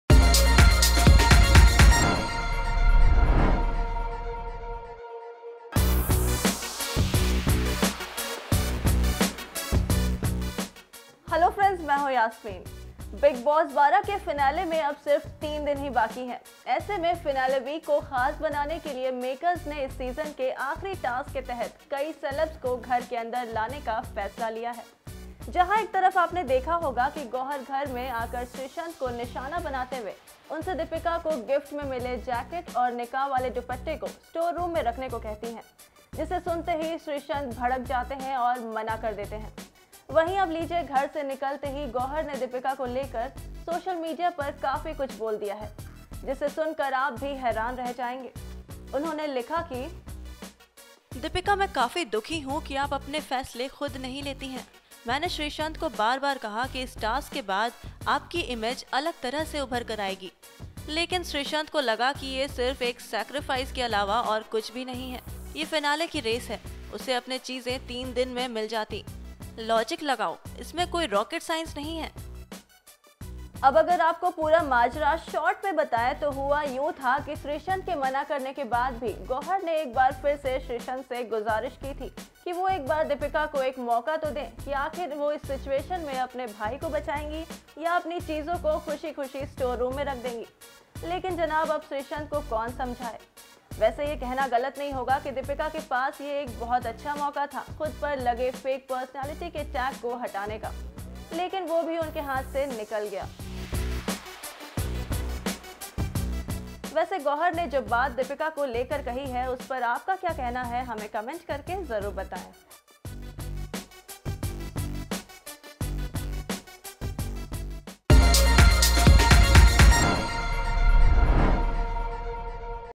Bigg Boss 12: गुस्से से लाल Gauhar Khan ने लगा डाली Dipika Kakar की वाट
जहां एक तरफ Bigg Boss में आपने देखा होगा की गौहर घर में आकर श्रीसंथ को निशाना बनाते हुए उनसे दीपिका को गिफ्ट में मिले जैकेट और निकाह वाले दुप्पटा को स्टोर रुम में रखने को कहती हैं जिसे सुनते ही श्रीसंथ भड़क जाते हैं और मना कर देते हैं...वही अब लीजिए घर से निकलते ही गौहर ने दीपिका को लेकर social मीडिया पर काफी कुछ बोल दिया है...जिसे सुन कर आप भी हैरान रह जाएंगे...